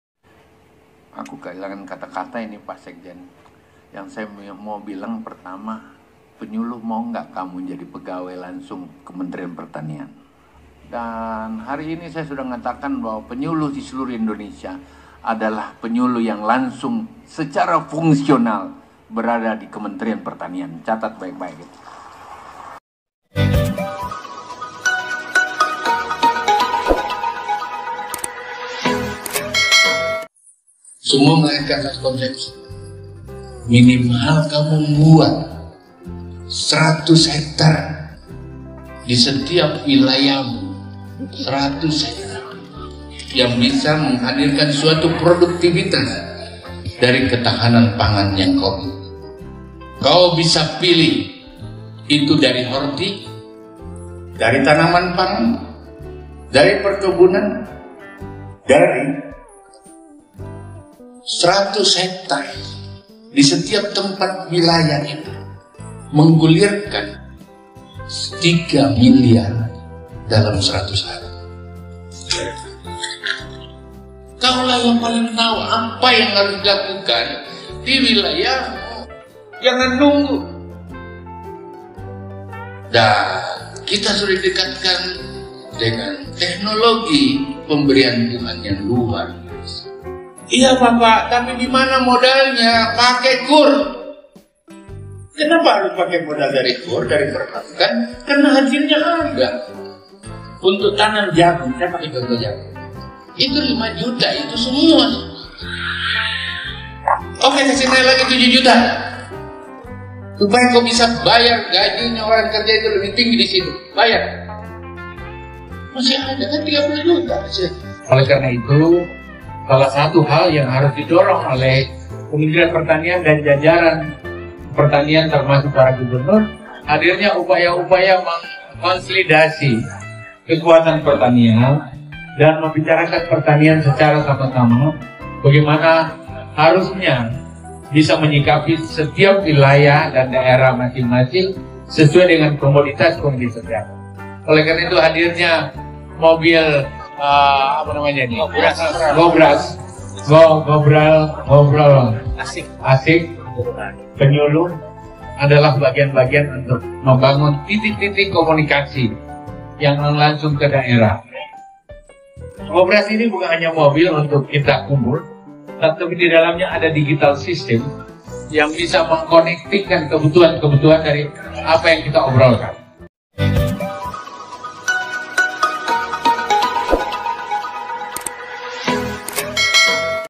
PENYULUH PERTANIAN LAPANGAN MENDAPAT MOTIVASI DARI MENTERI PERTANIAN SYL
Spirit Buat Penyuluh Pertanian Lapangan

Disampaikan pada Pembekalan Penyuluhan Pertanian Nasional pada hari Kamis (6/10/2022) di Auditorium Kementerian Pertanian RI